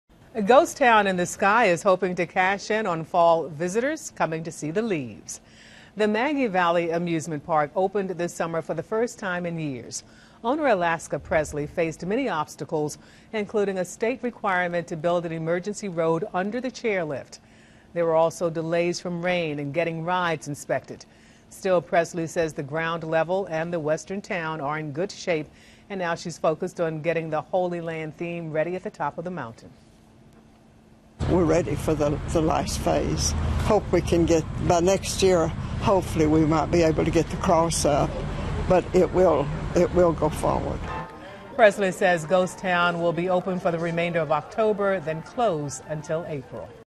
Ghost Town Open Through October
Ghost Town in the Sky is hoping to cash in on visitors coming to see Fall leaves.The Maggie Valley amusement park opened this summer for the first time in years. Owner Alaska Presley faced many obstacles including a state requirement to build an emergency road under the chairlift. There were also delays from rain and getting rides inspected.Still, Presley says the ground level, and the Western town, are in good shape. And now she's focused on getting the Holy Land themed, Inspiration Mountain ready at the top of the mountain.We're ready for the last phase, hope we can get by next year, hopefully we might be able to get the cross up.  But it will, it will go forward, says Presley.She says Ghost Town will be open for the remainder of October, then close until April.By Rex HodgeFollow Rex on Twitter @RexHodgeWLOS